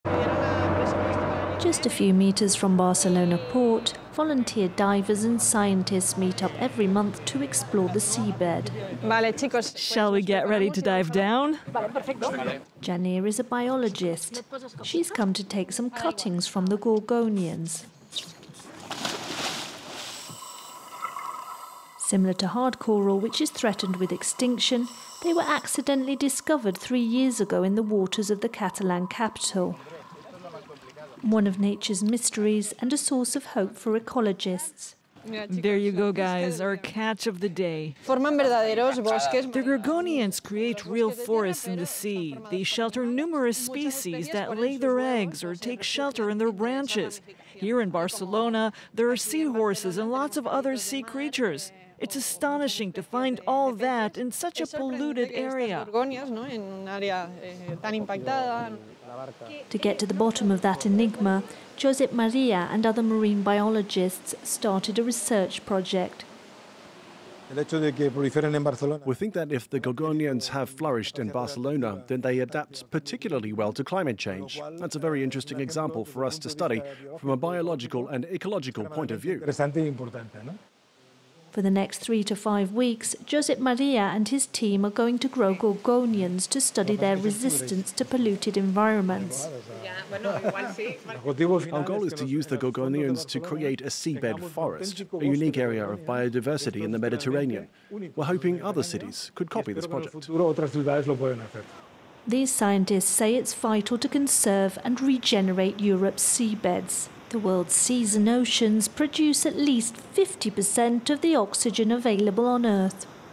Near the port of Barcelona, an amazing discovery to revitalise the seabed • FRANCE 24 English
Three years ago, scientists from the Institute for the Sea in Barcelona made an amazing discovery. They found gorgonians, a type of soft coral that is considered particularly sensitive to global warming, right near to the port of Barcelona, even though the area had significant pollution. Scientists decided to try using the discovery to revitalise the seabed. FRANCE 24’s team on the ground looked at their progress.